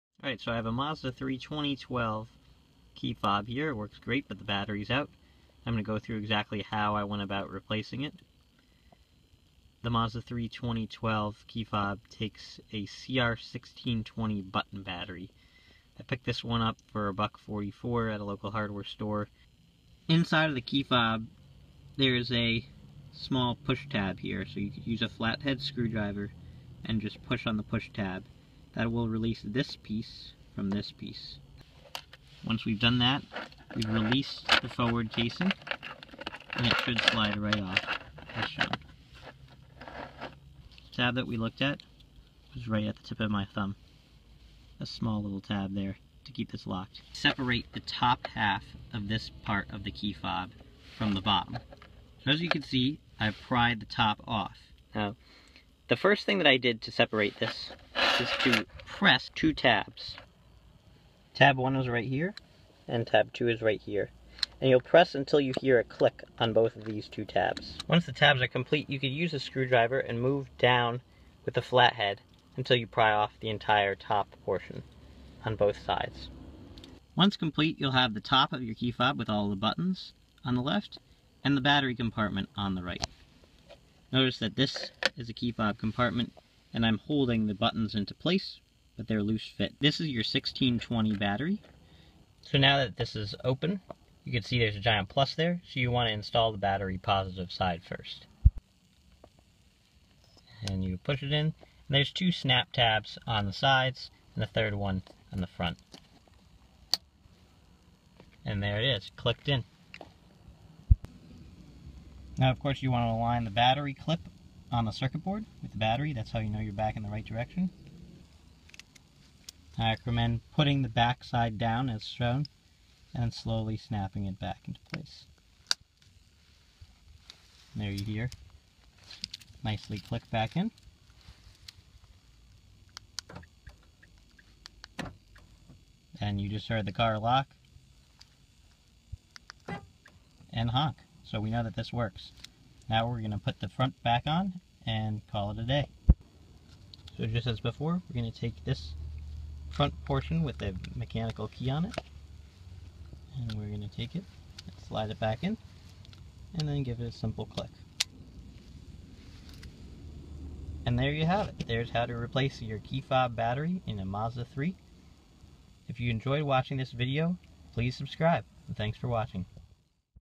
How to Change Mazda 3 Key Battery | Mazda 3 CX-3 CX-5 Mazda 6 | 3 Minutes
How to replace the battery in a Mazda Key Fob in under 3 minutes. The key shown is for a 2012 Mazda 3, but the replacement is similar for Mazda 3, 6, CX-3, CX-5, CX-7, and Miata.

Required Components:
Small Flat Head Screwdriver (under 1/8" wide with 1" length)
CR1620 Battery, purchased at a local hardware store for $1.44